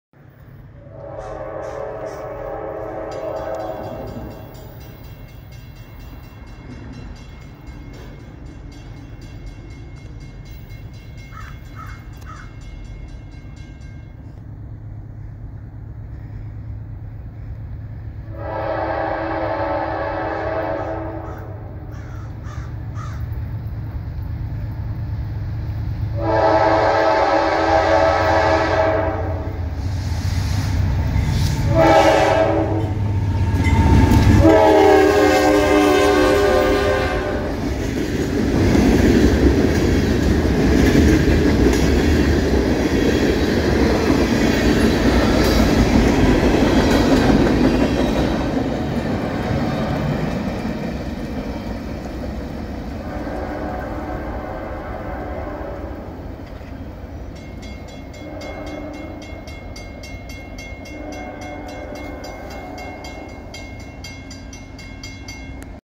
CSX LW14 Eastbound through Exeter Main Street 11/4/23
At 16:33, 11/4/23  a Lawrence - Worcester crew took a local from Lawrence Yard to Rigby yard after M427 was found without a crew, so the Lawrence yard crew ran east as "LW14" and then would later take M427 west from Rigby.